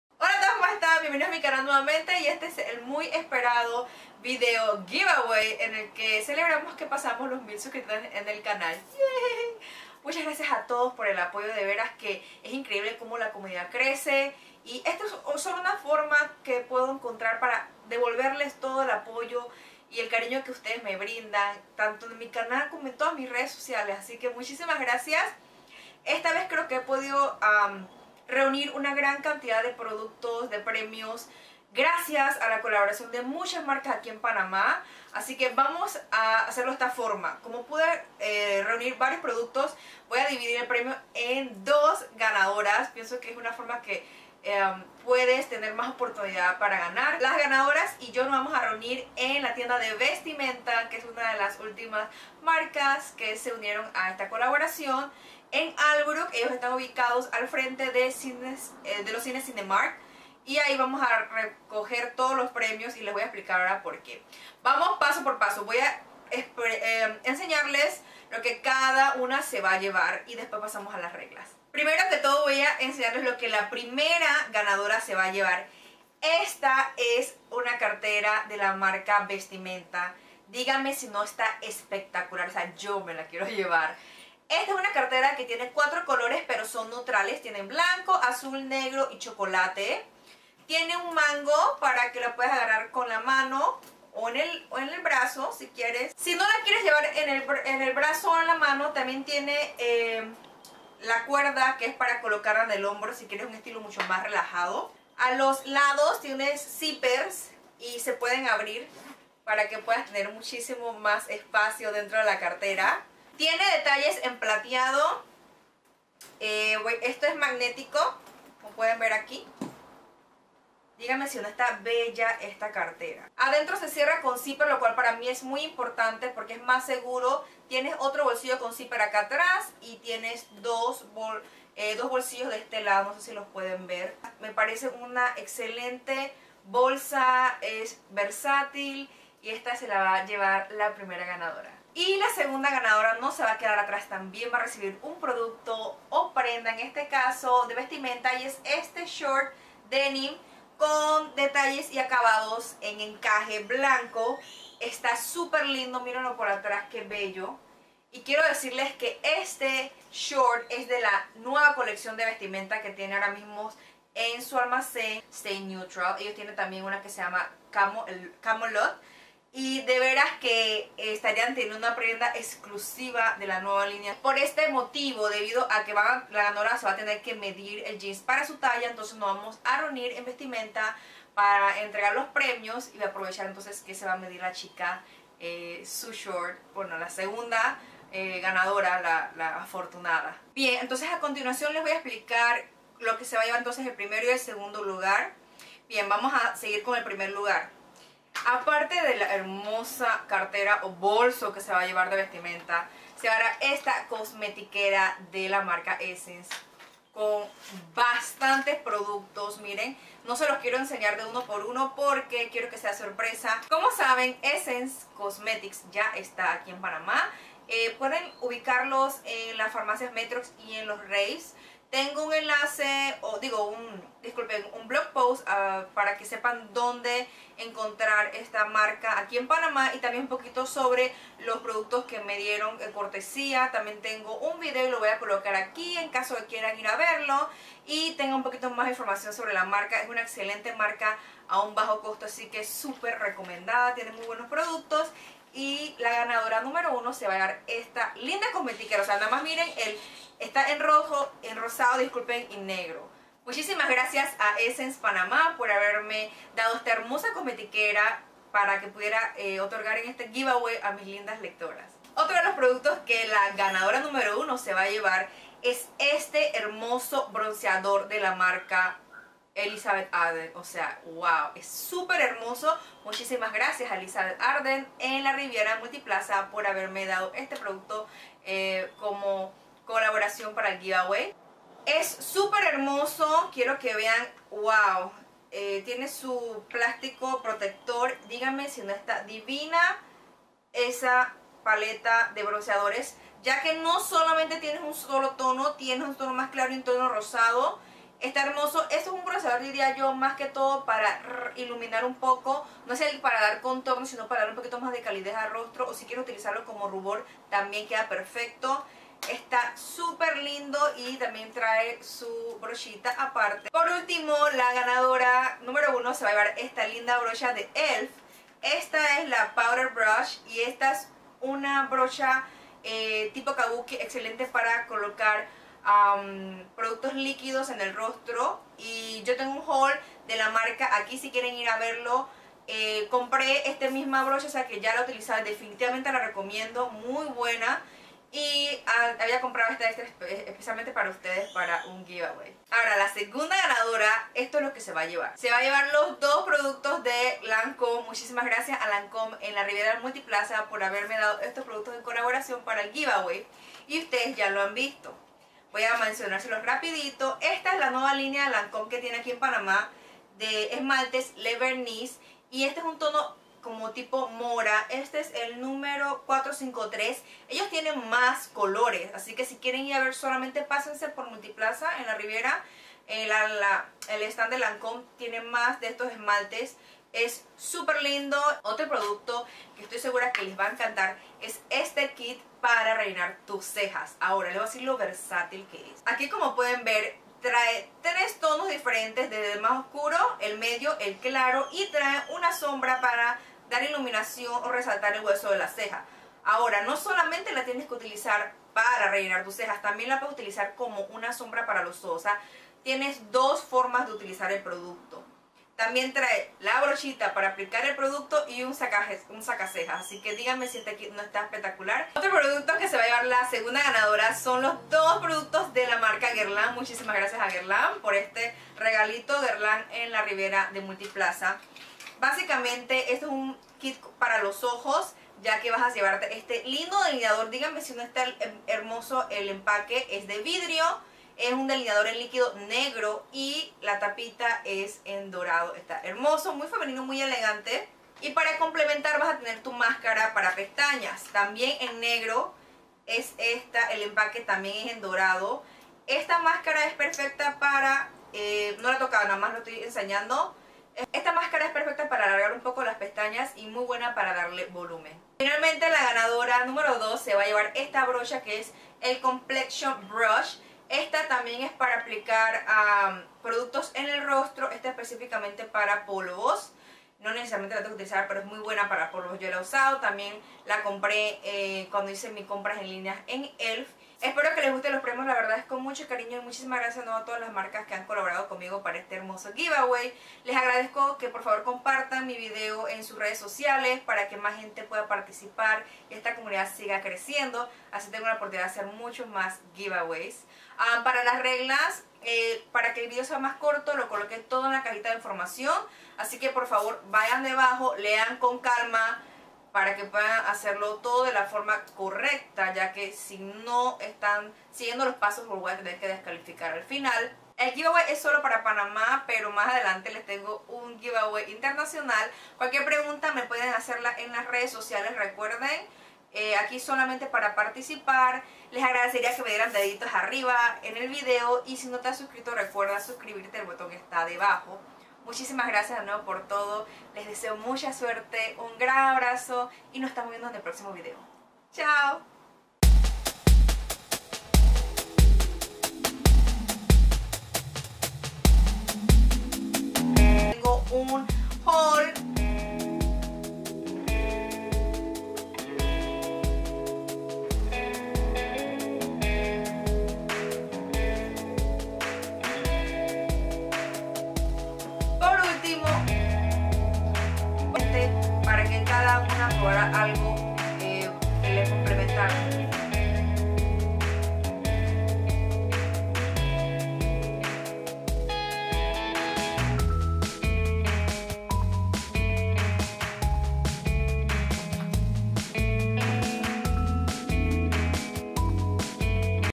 1,000 Subscribers Giveaway solo Panamá (CERRADO)
ABREME
Hola mis bellezas,
Este es el giveaway que les había prometido finalmente, para celebrar que pasamos los 1,000 suscriptores en el canal de youtube, muchas gracias por todo el apoyo.

Pasemos a las reglas:
-Solo para Panamá (Nacional)
-Mayores de 18 años
-Dejar un comentario (USANDO LA CUENTA CON LA QUE SIGUEN MI CANAL), diciéndome cuál de los 2 premios quisieras ganarte y porqué (RECUERDEN DEJAR SU PERFIL DEL CANAL PUBLICO PARA VERIFICAR QUE ME SIGUEN).

Pueden participar cuantas veces quieran hasta que termine el giveaway, PERO PROCUREN NO POSTEAR TAN SEGUIDO DE LO CONTRARIO YOUTUBE LO CONSIDERARA SPAM Y NO SERA TOMADO EN CUENTA.

Las ganadoras deben tener disponibilidad para reunirse conmigo un fin de semana (preferiblemente Domingo) para recibir los premios en la tienda Vestimenta en Albrook Mall.
El giveaway durará desde 11 de marzo hasta 28 de marzo, tienen para participar hasta las 12 media noche del 28 de marzo.
Las ganadoras serán escogidas al azar usando Random.org (se verificarán los pasos)
Muchas suerte a tod@s

Instagram (si no tienes celular, puedes seguirme en línea) Si tienes Instagram estoy como: IleanaRecommends